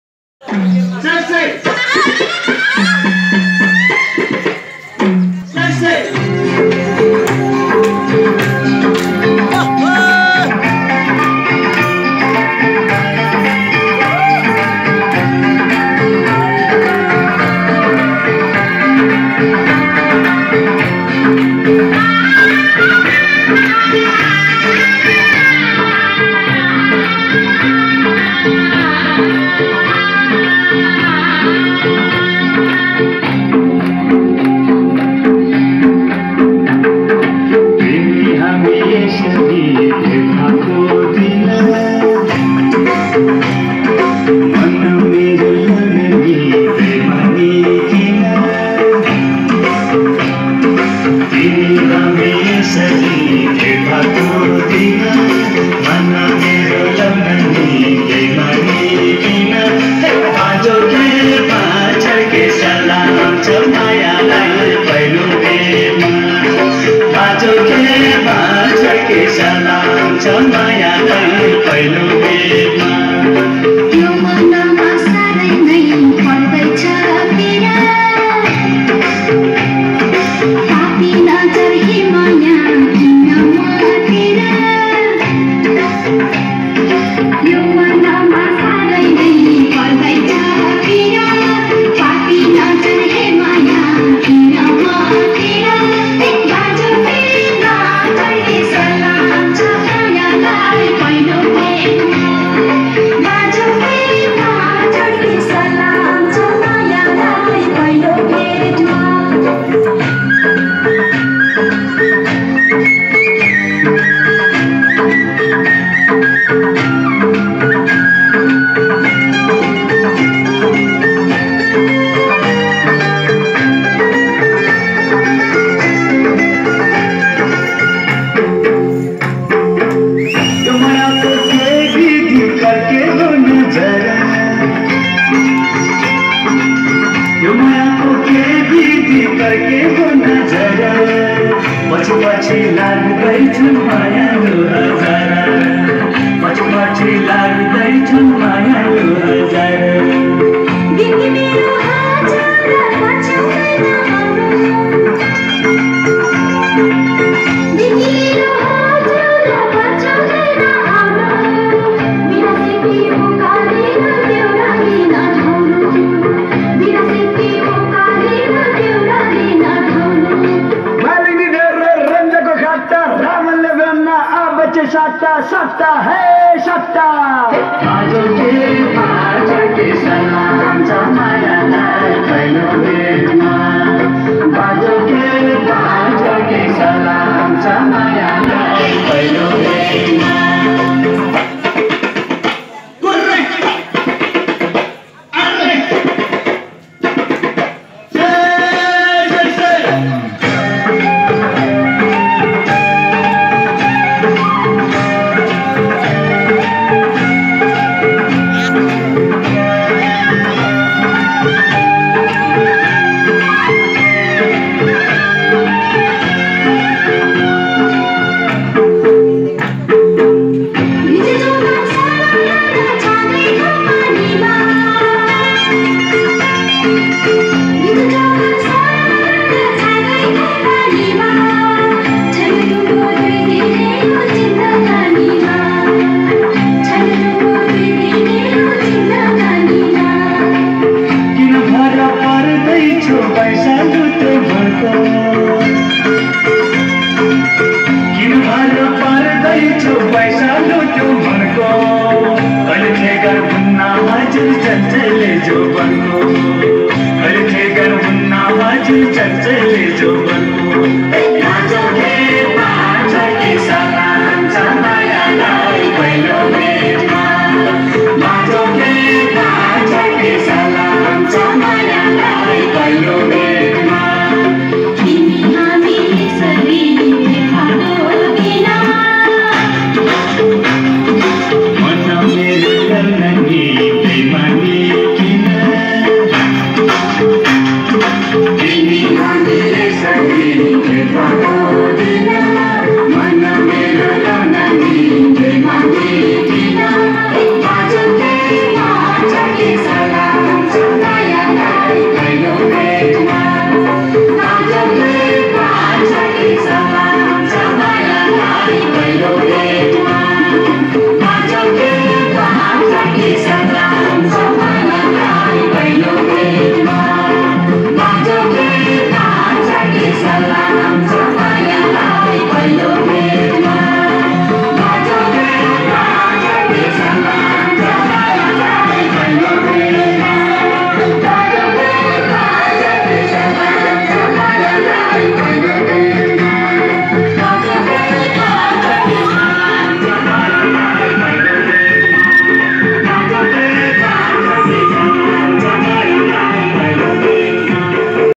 Deusi vailo dance (jharana & aawas)
When i showed to my friends, relatives ... they said it felt good to watch these two kids dance ... so i felt to share to the world ...